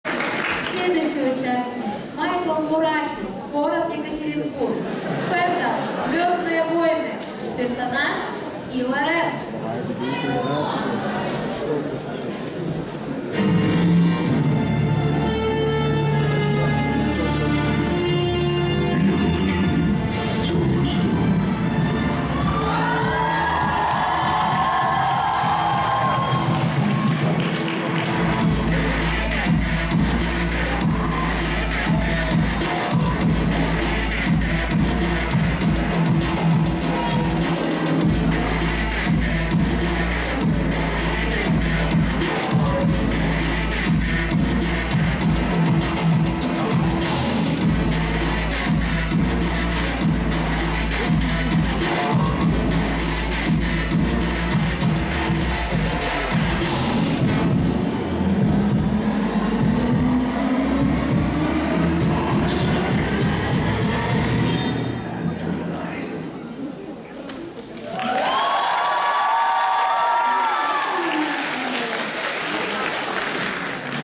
M.O.con 2016: Косплеер Майкл Мураки - Kylo Ren (Star Wars)
Фестиваль M.O.con 2016 (Декаданс) - Одиночное дефиле Запад
Косплеер: Майкл Мураки (Екатеринбург)
Фэндом и персонаж: Kylo Ren (Star Wars: The Force Awakens)